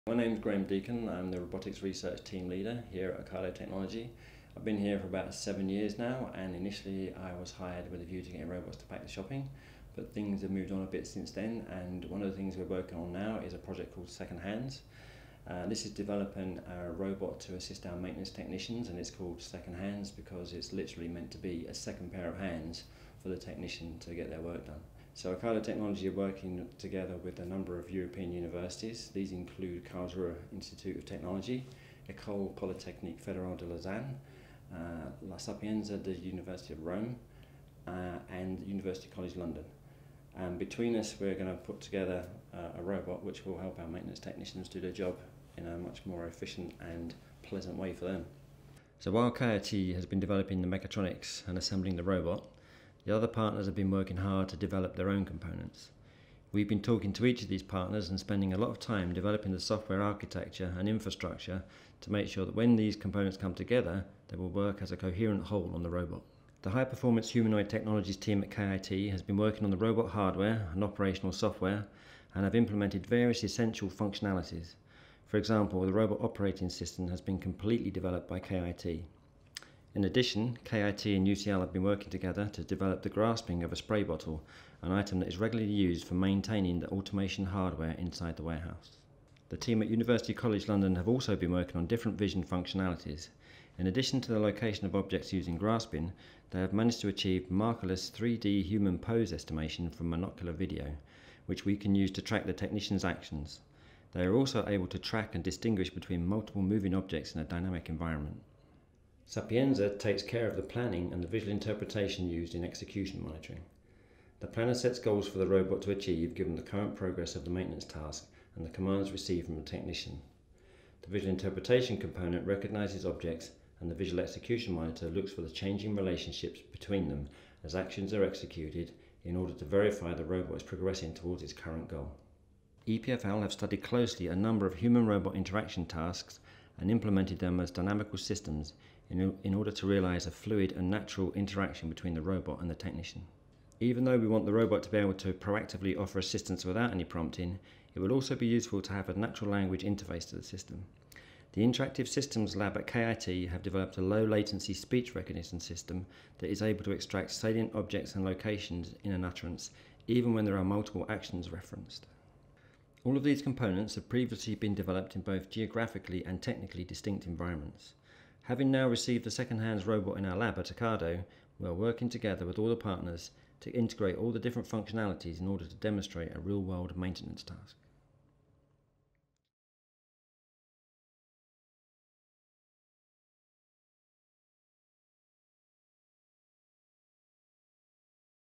SecondHands project members present first robot prototype ARMAR-6
SecondHands is an EU-funded Horizon 2020 project aiming to design a collaborative robot (cobot) that can proactively offer support to maintenance technicians working in Ocado’s highly automated warehouses, also known as Customer Fulfilment Centers (CFCs). This robot will be a second pair of hands that will assist technicians when they are in need of help. The robot will learn through observation and will augment the humans' capabilities by completing tasks that require a level of precision or physical strength that are not available to human workers.